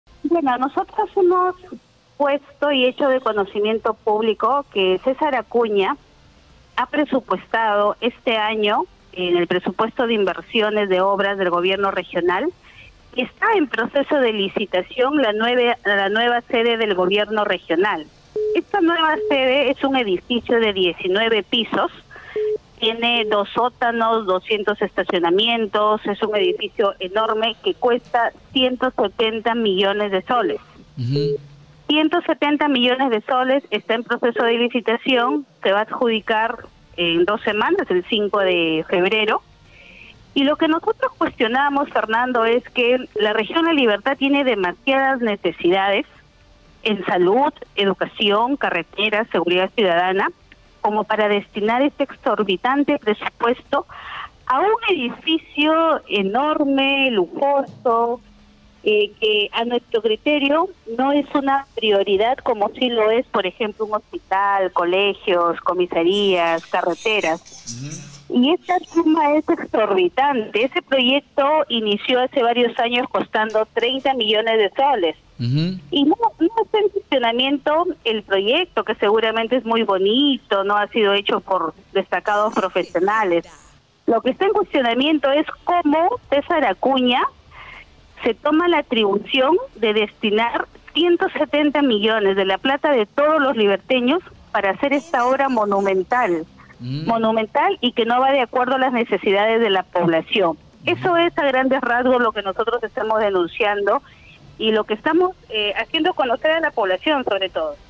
EX VICEGOBERNADORA CUESTIONA A GORE QUE GASTARÍA S 170 MILLONES EN CONSTRUIR NUEVO LOCAL
🎙️N60Noticias| Exvicegobernadora de La Libertad, Mónica Sánchez, indicó en N60Radio que millonario presupuesto del Gobierno Regional podría utilizarse en carreteras, seguridad ciudadana u hospitales. Buena pro se dará el 5 de febrero, según la OSCE.